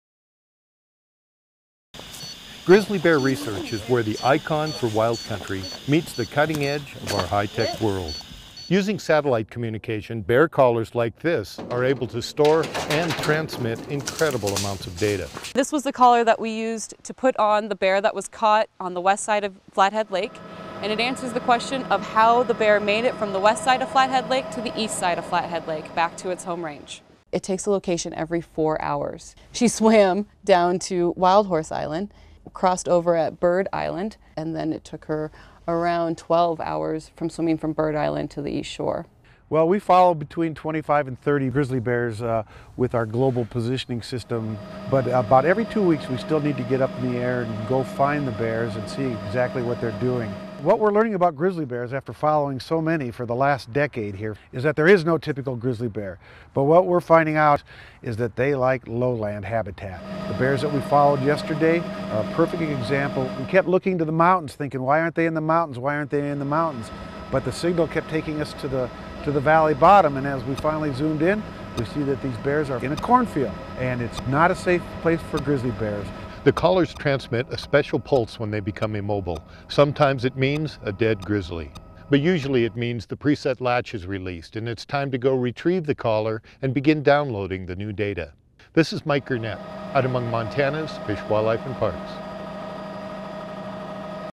GPS Griz
Technology allows researchers to learn more about Grizzly bears.